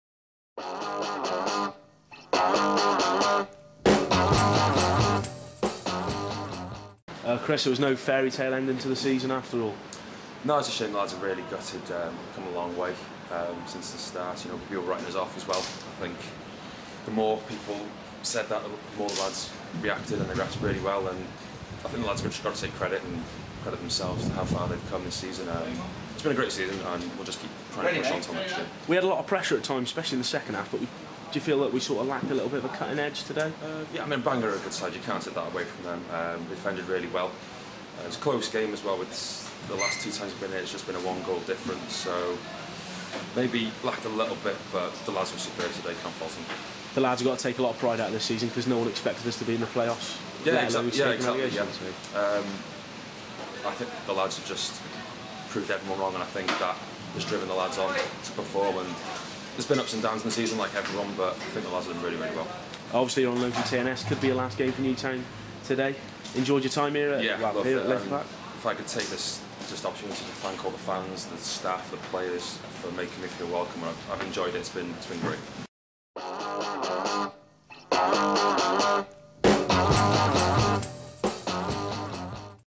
Chris Mullock Bangor City Post match interview
On loan keeper Chris Mullock praises the fans and staff for the support he has received in what could be his last game for the club.